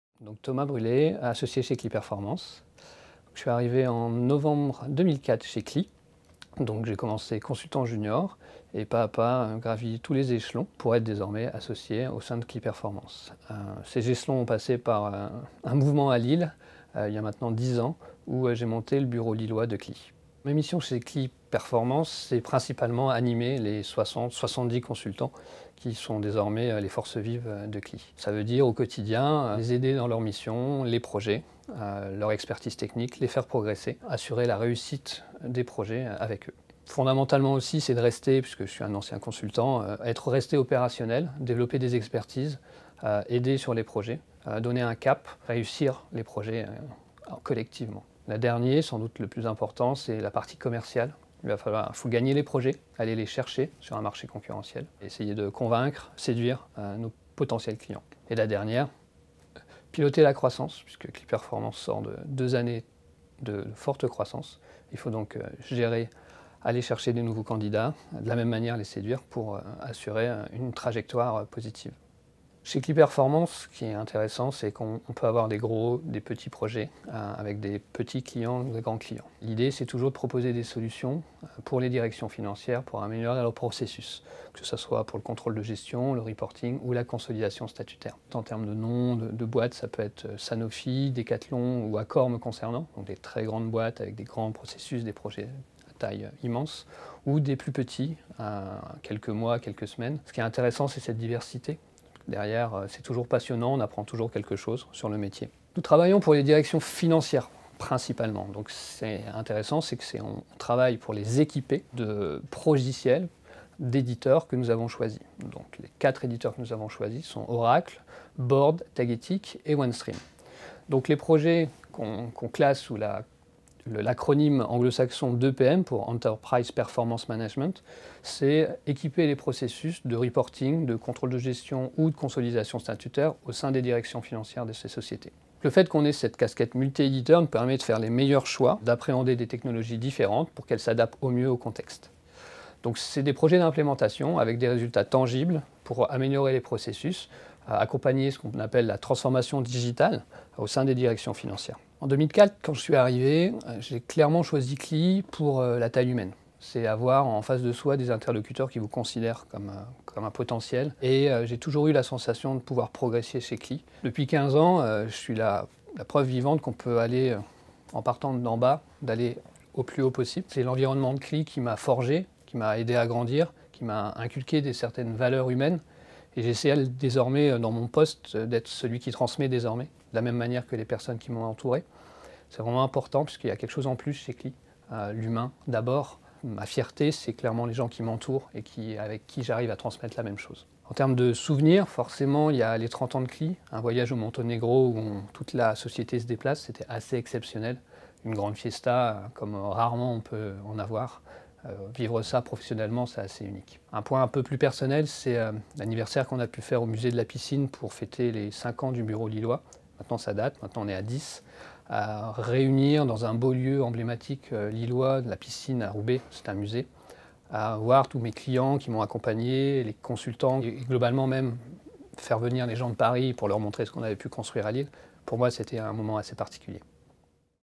Découvrez Klee Group avec Thomas, Directeur Associé de Klee performance
Klee Group recrute !